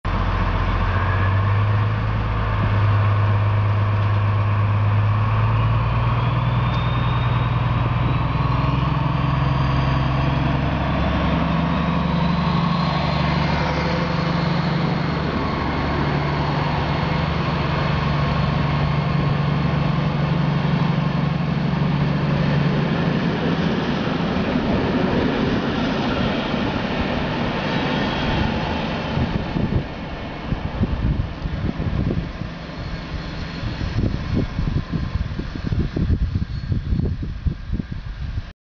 DB 218.241 Kaufbeuren
DB 218.241 leaves Kaufbeuren with a regional train from München - Lindau